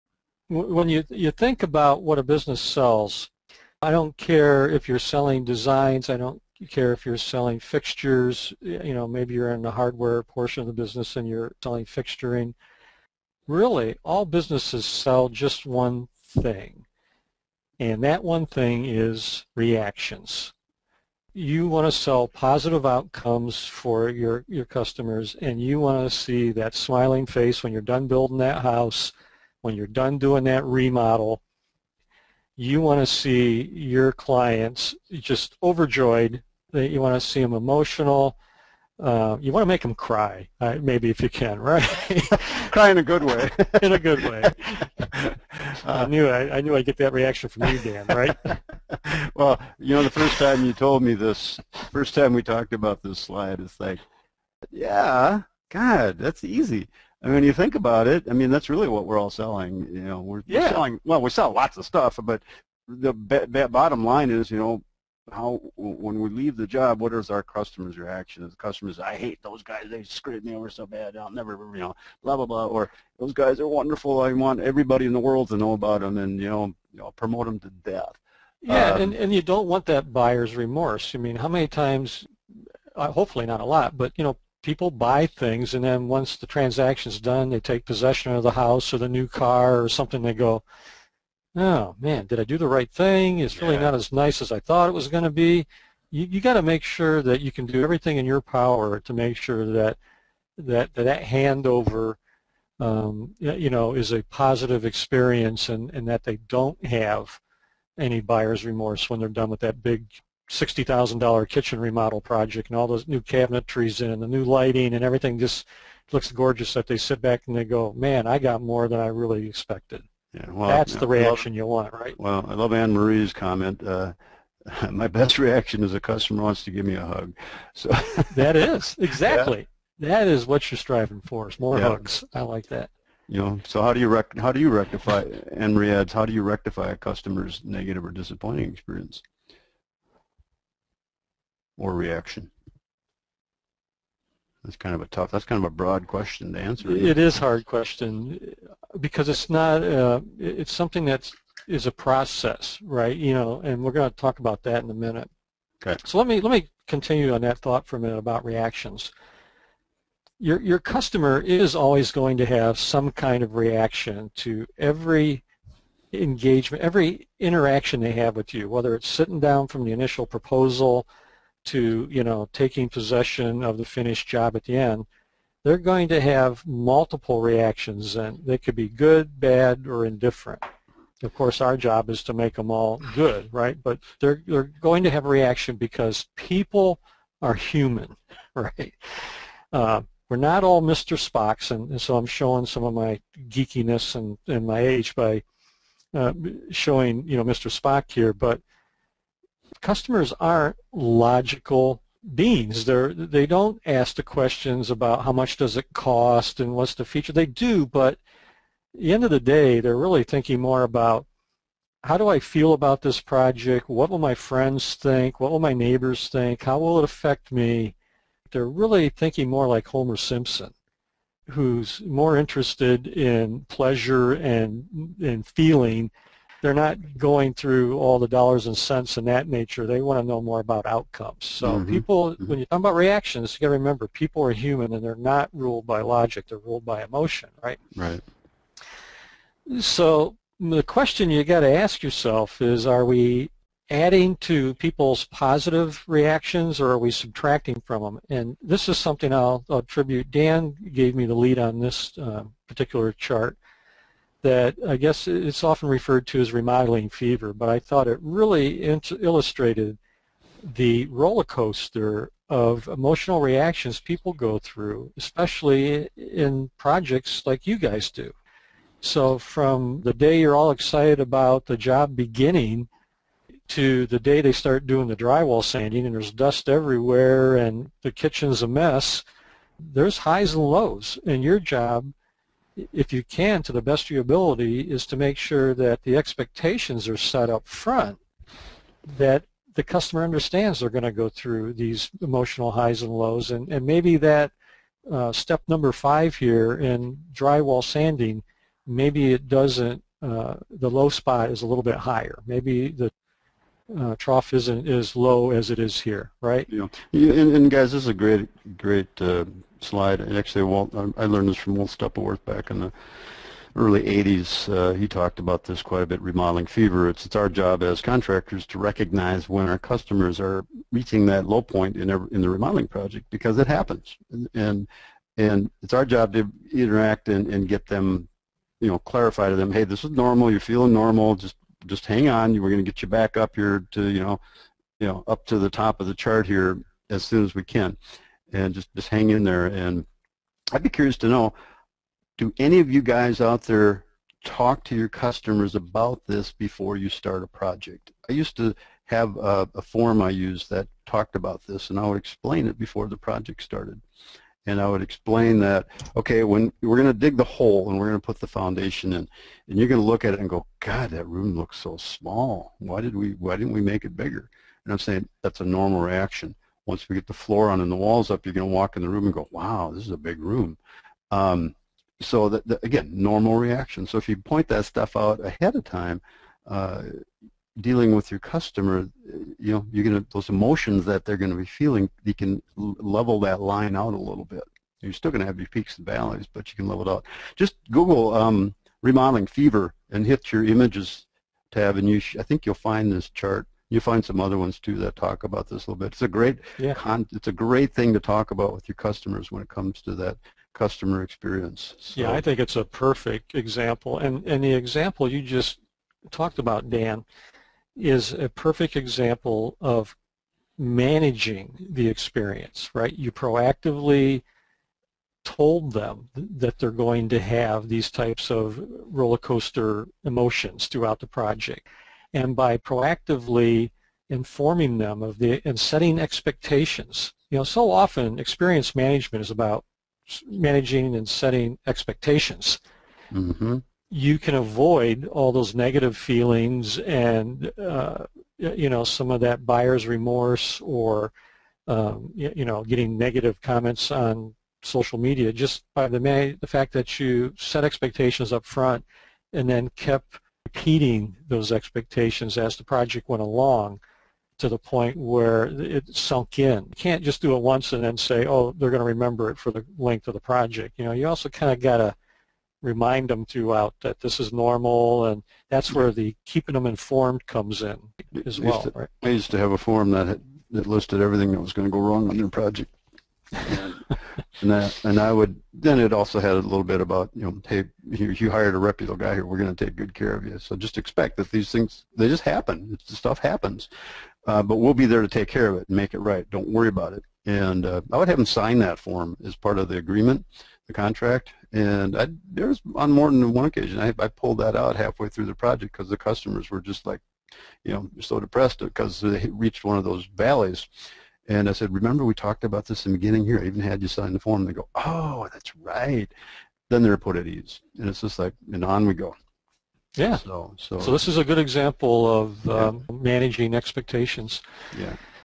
Customer Experience Emotional Chain Reactions - Part 2
interviews Customer Experience Marketing expert John Weisenberger.  Learn why you want to make your construction design and contracting customers cry plus how to manage your customer's emotional "Roller Coaster" often experienced during remodeling projects.